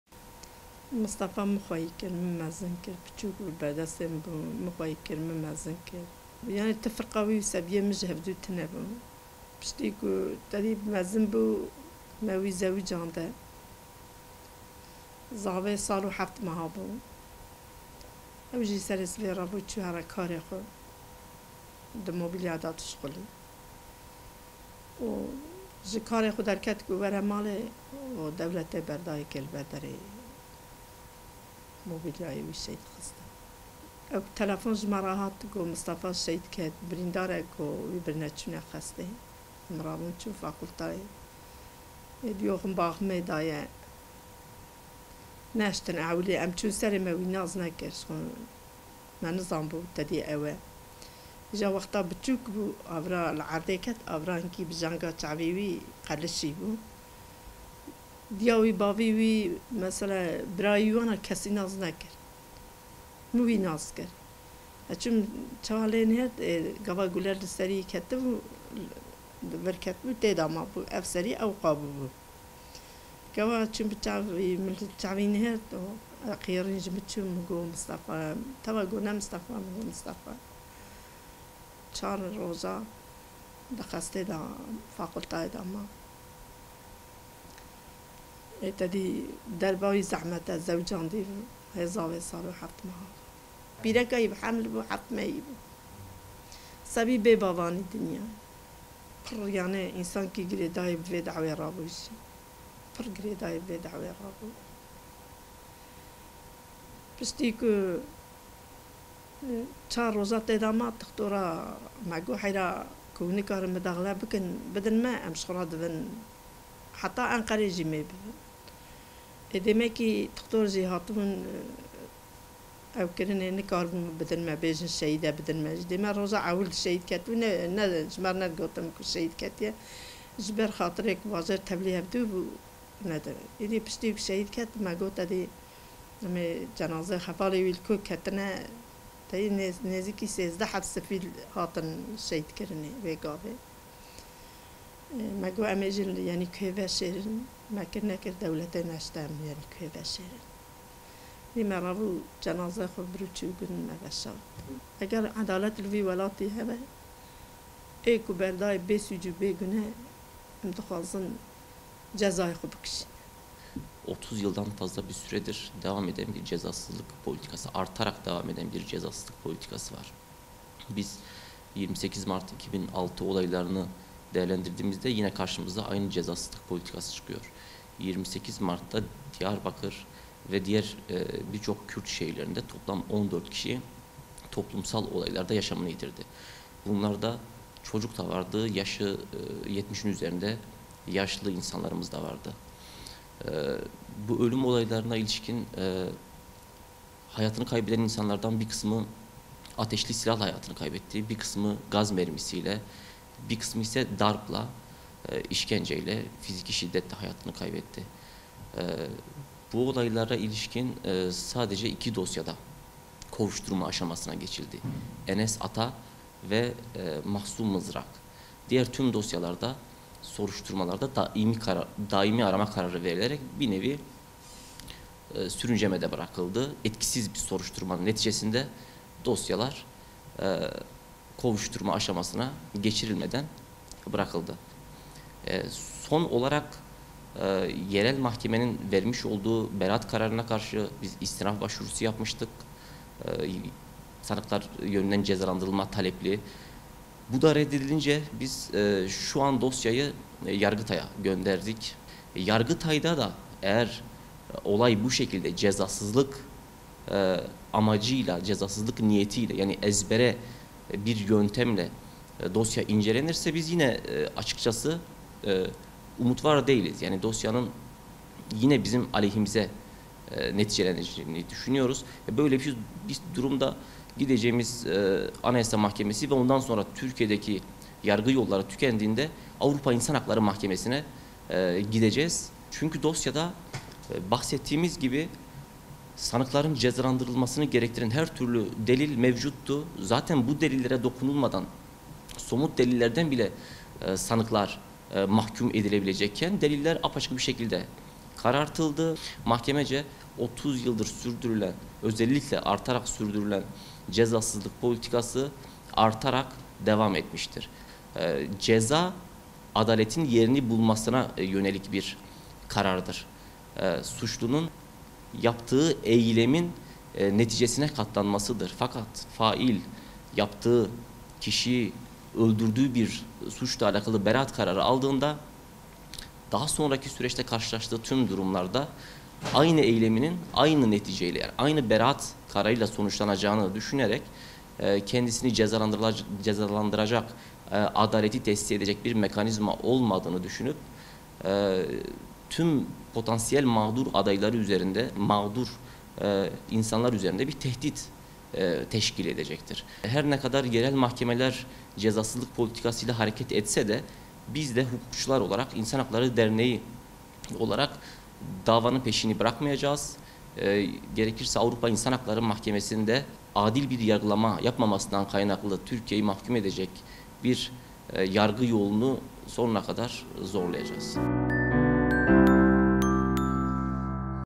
28 MART 2006 OLAYLARININ FAİLLERİ 15 YILDIR CEZASIZ
Muş'un Şenyayla kırsalında 24 Mart 2006 tarihinde 14 HPG'linin kimyasal silahlarla öldürülmesi ardından 4 HPG'li için 28 Mart'ta Diyarbakır'da cenaze töreni düzenlendi. On binlerce kişinin katıldığı yürüyüşle Yeniköy Mezarlığı'nda cenazeler toprağa verilirken, tören sırasında çok sayıda F-16 mezarlık üzerinde uçuş yaptı. Bağlar 10 Nisan Polis Karakolu önünde yoğun önlemler alan polisler, mezarlıktan dönen kitleye zırhlı araç, gaz bombası ve tazyikli suyla müdahale etti. Müdahale ile olaylar kısa sürede Diyarbakır’ın geneline yayıldı. Ardından protestolar Batman, Mardin'in Kızıltepe ve Nusaybin ilçeleri, Şırnak, Dersim, Van, Siirt, Urfa merkez ve Ceylanpınar ile Viranşehir ilçeleri olmak üzere birçok il ve ilçeye sirayet etti. Olaylar devam ettiği sırada dönemin Başbakanı Tayyip Erdoğan'ın, "Terörün maşası haline gelen her kim olursa olsun, kadın da olsa çocuk da olsa gereken yapılacaktır" açıklamasıyla polis ve askerin saldırısında 7’si çocuk 14 kişi yaşamını yitirdi, çok sayıda kişi de yaralandı.